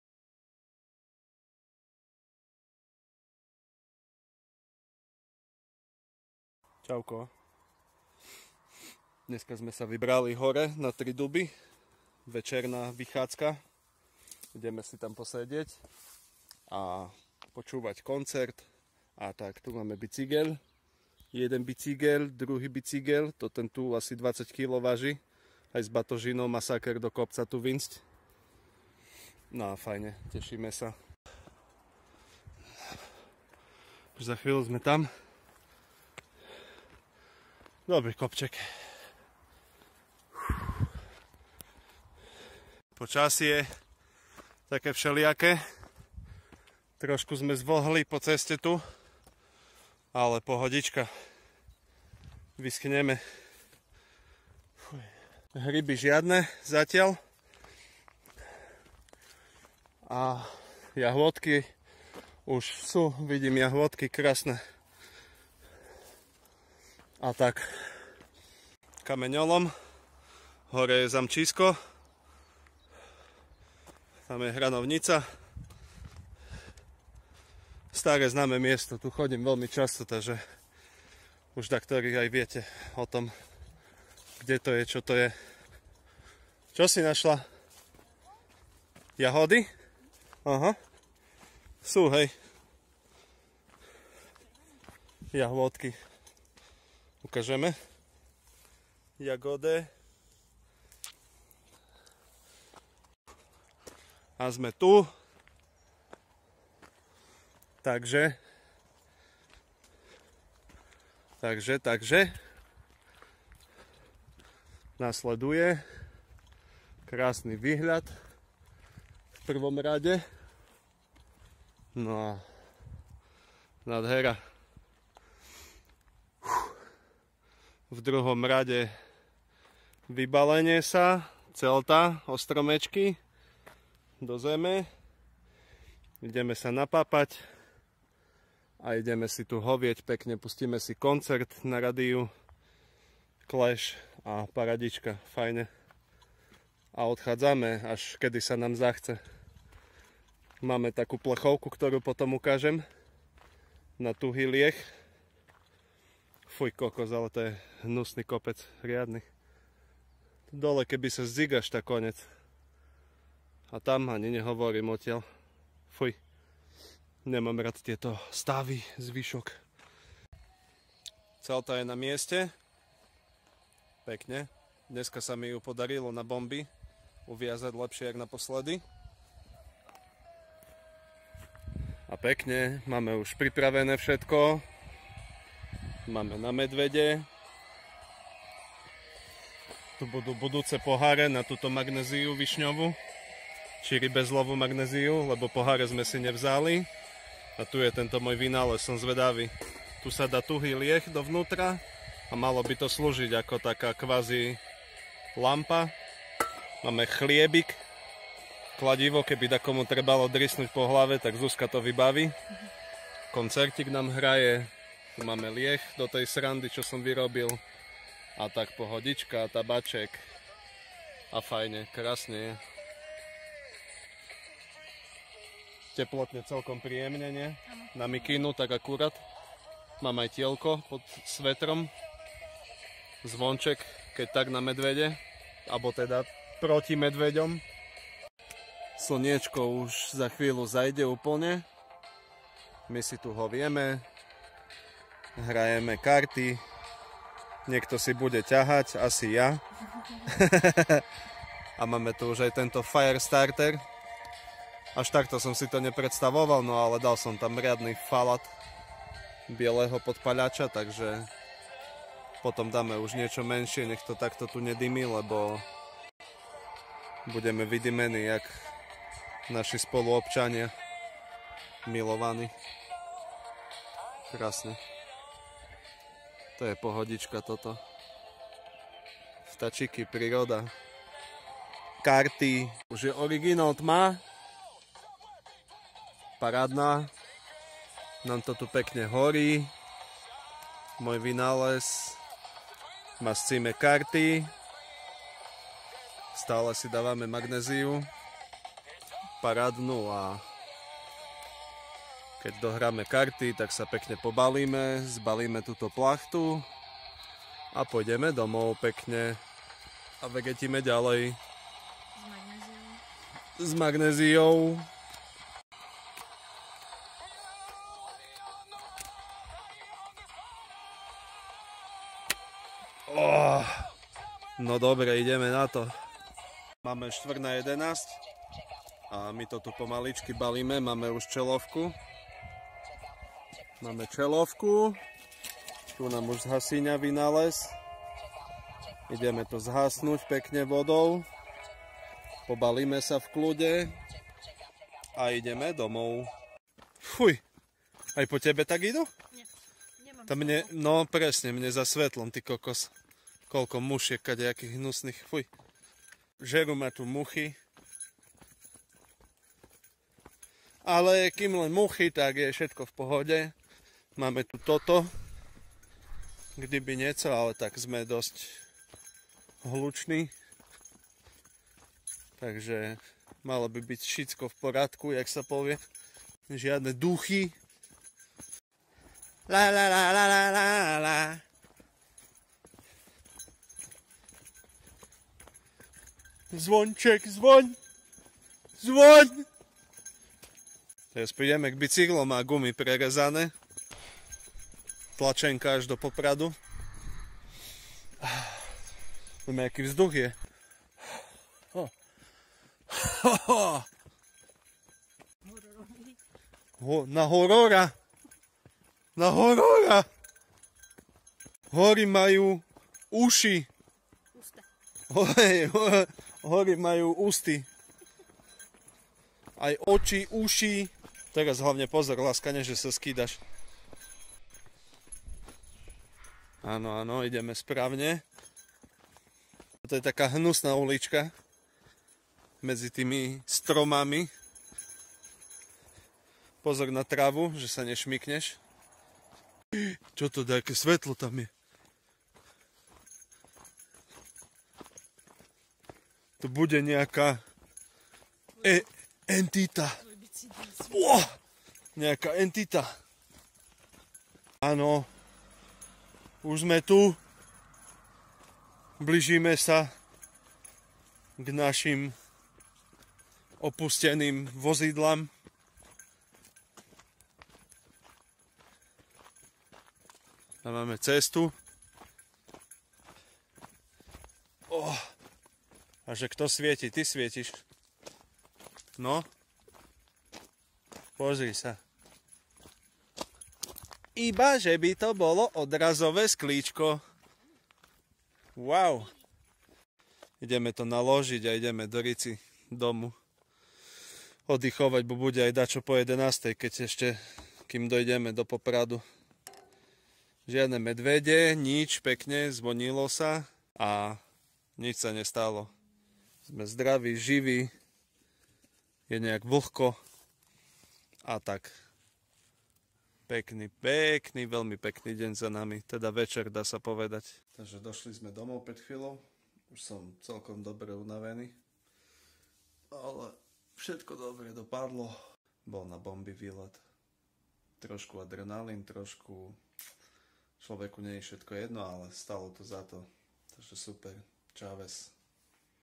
TRI DUBY VEČERNE POSEDENIE VO DVOJICI
Večerné romantické posedenie zo 4.júla tohto roka vo dvojici, na legendárnom mieste nad Kvetnicou. Po príchode domov sme zistili že sme vďaka môjmu vynálezu na tuhý lieh ako dve vyudené klobásy. Každopádne pekný výjazd vo veľmi príjemnej spoločnosti.